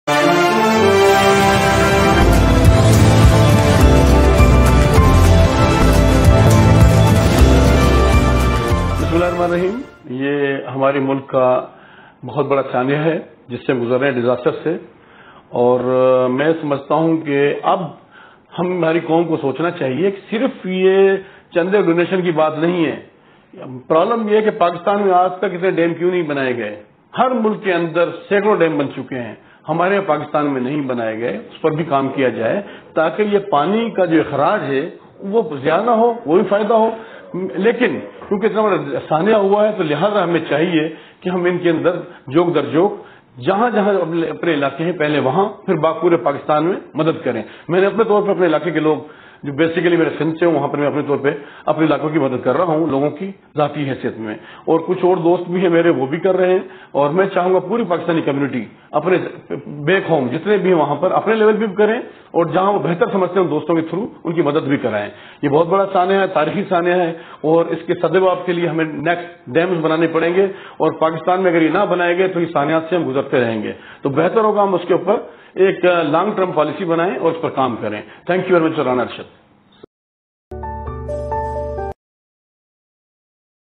Makhdoom Raees Qureshi Advocate || talks about flood in Pakistan || emaratnews
دبئی ۔ امارات کے ممتاز قانون دان مخدوم رئیس قریشی نے کہا کہ وقت آ گیا ہے کہ ہم ایک قوم بن کر سوچیں اور آنے والی نسلوں کو سیلاب جیسی آفات سے محفوظ کرنے کیلئے مزید ڈیمز تعمیر کریں  ۔ اس کے ساتھ ساتھ دریاوں ۔ نہروں اور ندی نالوں کی بھل صفائی اور ان کی مزید گہرائی ایک ریگولر پروگرام کے تحت جاری رکھیں  ۔
مذید کیا کہا انہوں نے ۔ آئیے دیکھتے ہیں ۔

ارشد محمود رانا ۔
امارات نیوز ۔
دبئی ۔
متحدہ عرب امارات ۔